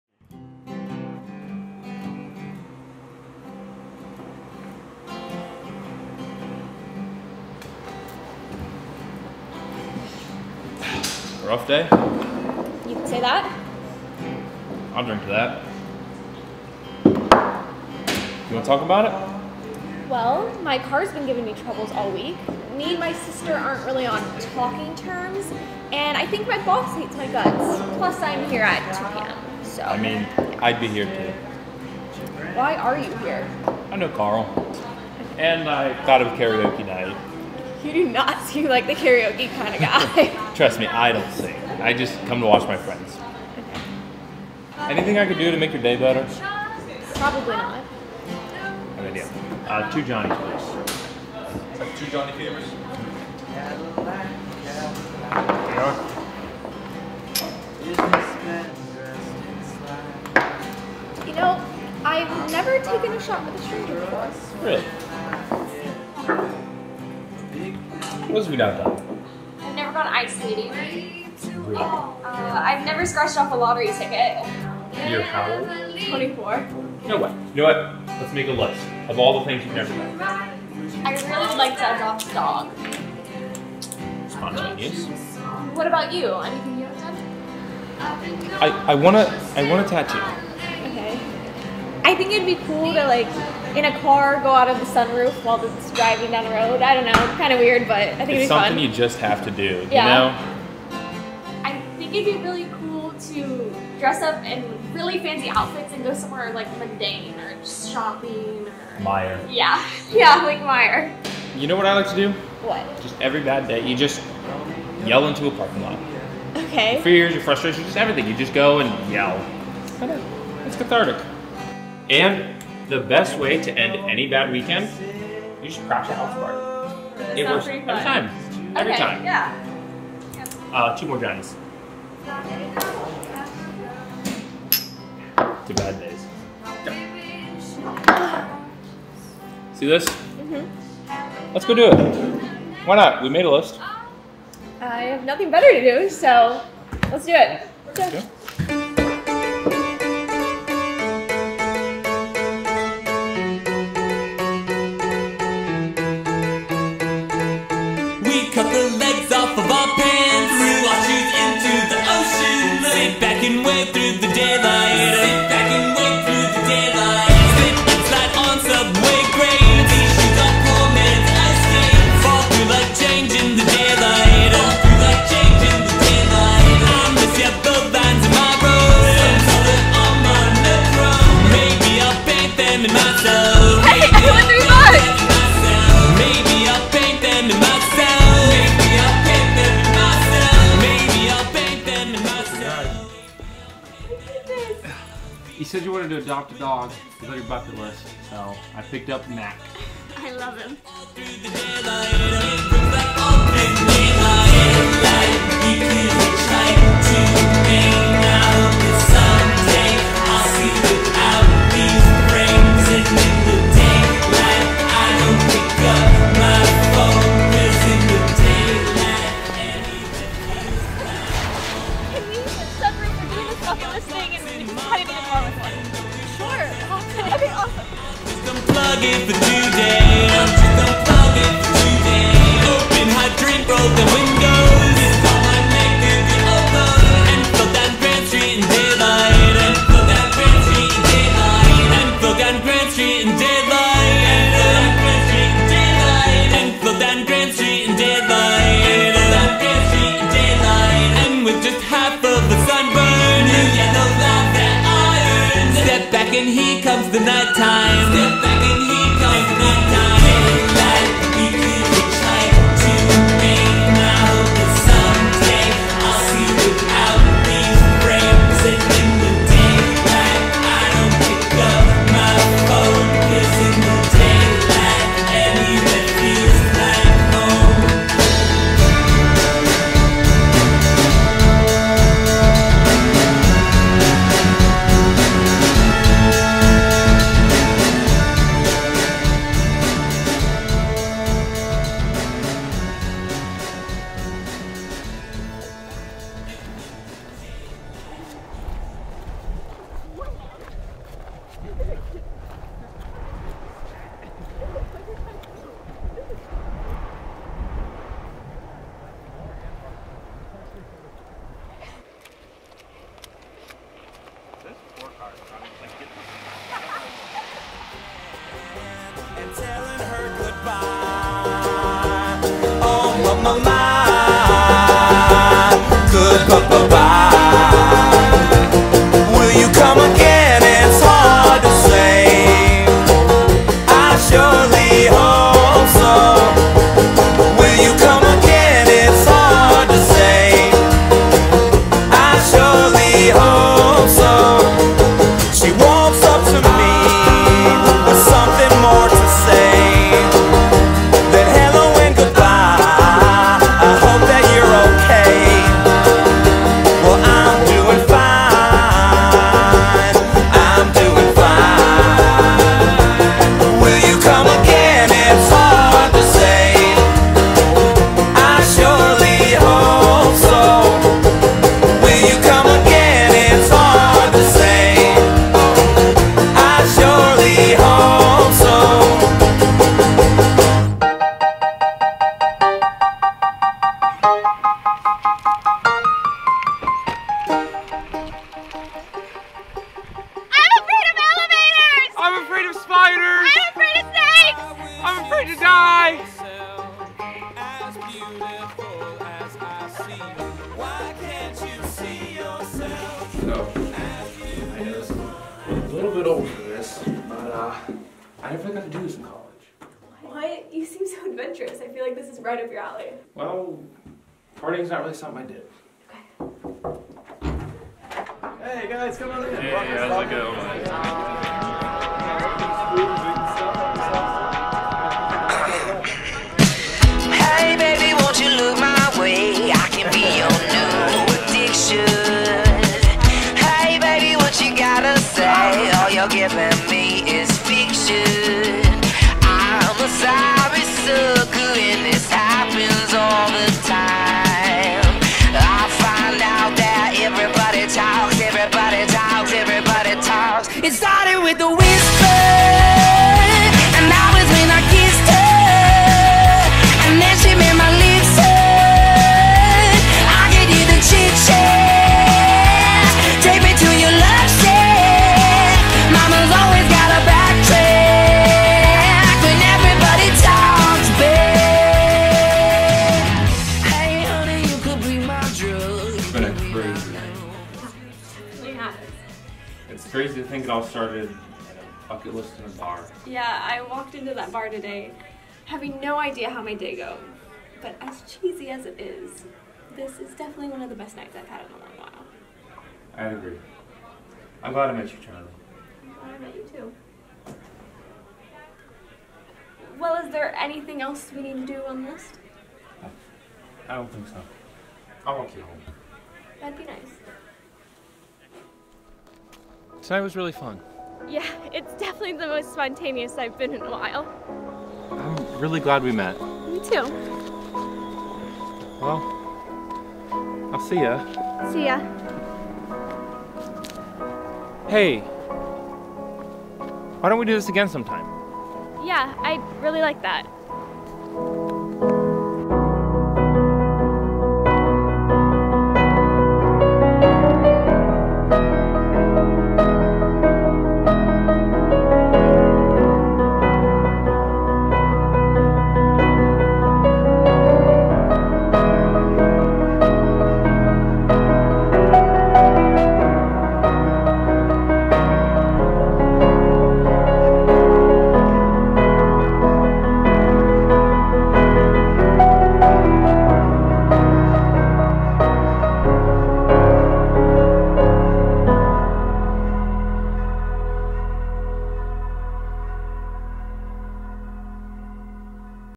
The Night We Met
A short film that we made in place of the 48 hour film festival. Enjoy!